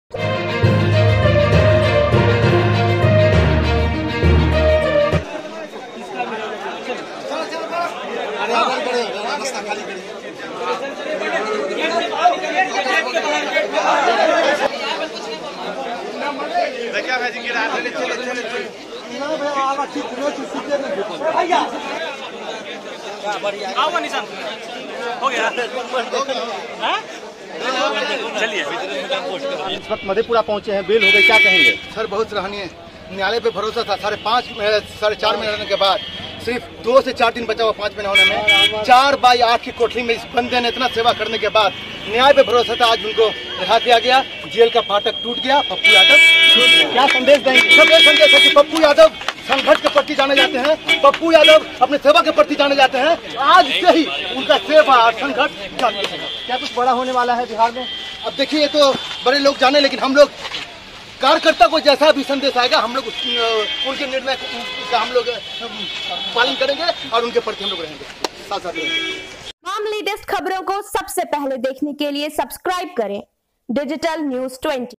32 साल पुराने अपहरण के केस में जाप सुप्रीमो पप्पू यादव रिहा|Released after five months
डिएन 20, मधेपुरा/दरभंगा : जन अधिकार पार्टी के राष्ट्रीय अध्यक्ष और पूर्व सांसद राजेश रंजन उर्फ पप्पू यादव को अपहरण के एक 32 साल पुराने केस में मधेपुरा कोर्ट ने सोमवार को रिहा कर दिया। इस खबर के बाद जाप कार्यकर्ताओं में खुशी की लहर है। पप्पू यादव ने न्यायालय और आम जनता का आभार जताया है।
पप्पू यादव पिछले 5 महीने से न्यायिक हिरासत में दरभंगा के डीएमसीएच के मेडिसिन आईसीयू में इलाजरत थे। बता दें कि इस केस में मधेपुरा कोर्ट ने उन्हें सुपौल के वीरपुर जेल भेजा था। जहां तबीयत बिगड़ने के बाद उन्हें इलाज के लिए दरभंगा के डीएमसीएच में भर्ती कराया गया था।

:21
02igshid:12m6qc65f6r02
20/pref:bookmarks